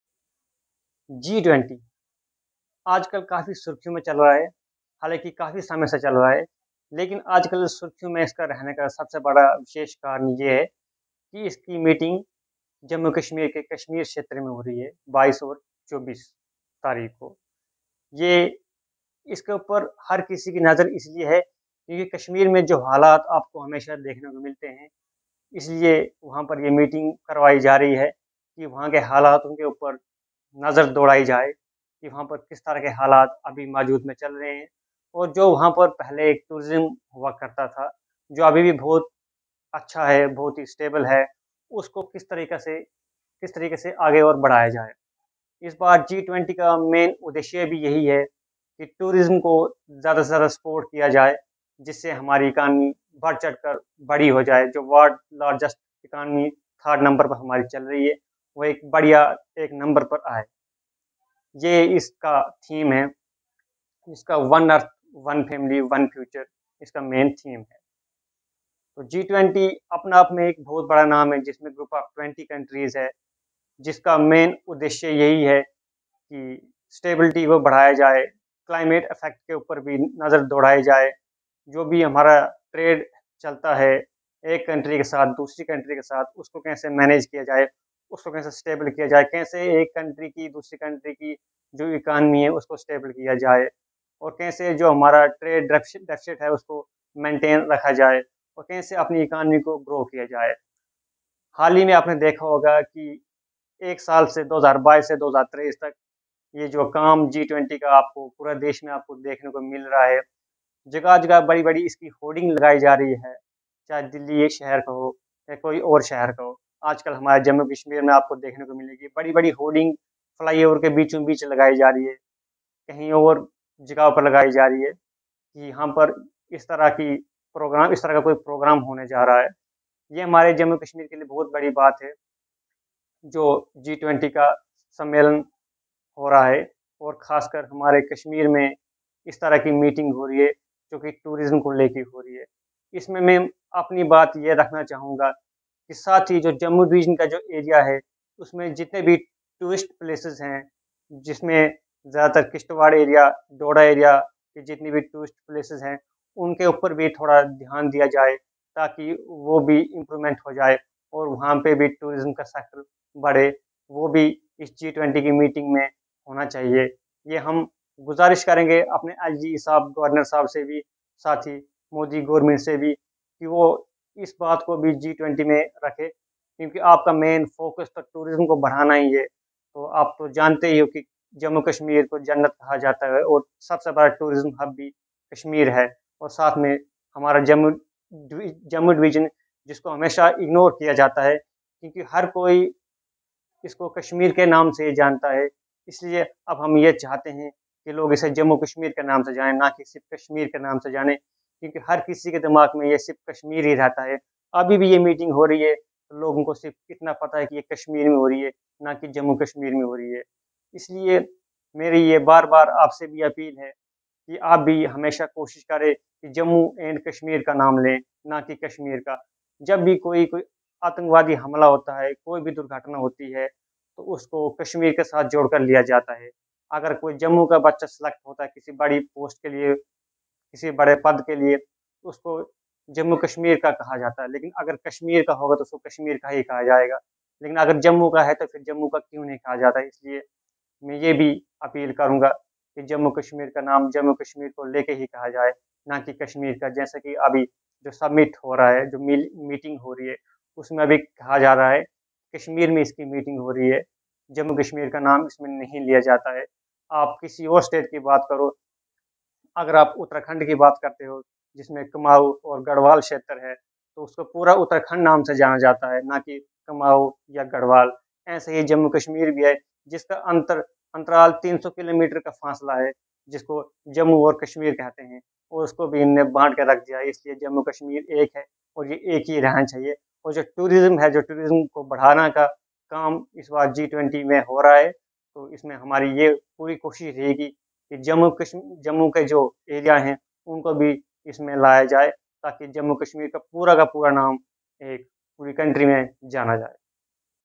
G20 Summit Kashmir May Jammu Kashmir Ka Name Kyu Nahi Bola Jata
Officials said the idea of the film tourism event is to showcase film shoot offerings to the visiting delegates with a focus on single-window clearance and other facilities to foreign crews. The third G20 tourism working group meeting under India's presidency will be held in the Kashmir Valley between May 22 and 24.